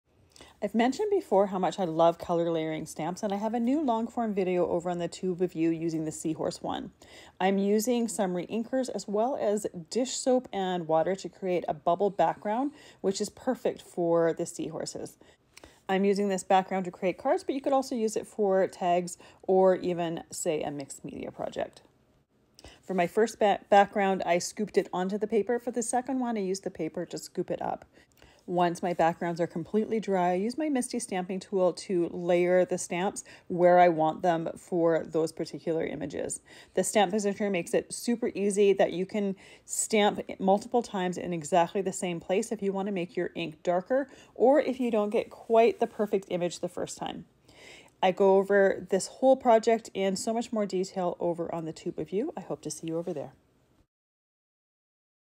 Bubble Background Tutorial
In this video, I'm sharing a Bubble Background Tutorial.  This Bubble background is a Fun (but a little Messy) way to create a unique background behind this Seahorse Image.  The colour used can be customized to your liking and is perfect for Beachy Themed cards or Projects.

Full Supply list with clickable links in Related Video description